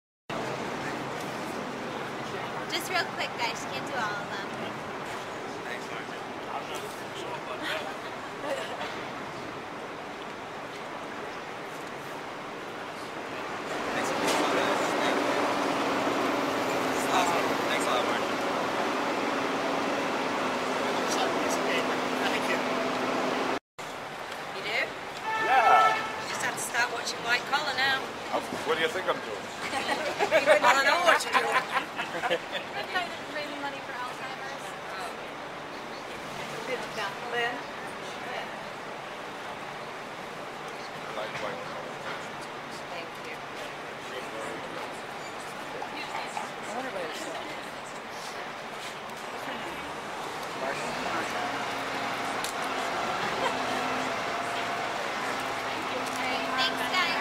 Marsha Thomason - Signing Autographs at "White Collar" Q & A in NYC
Marsha Thomason signed autographs at a "White Collar" screening and Q & A in New York City. For authentic autographed memorabilia of Marsha Thomason and many other celebrities, check out TopPix Autographs Get the Best Deals on Authentic Autographs on our eBay Store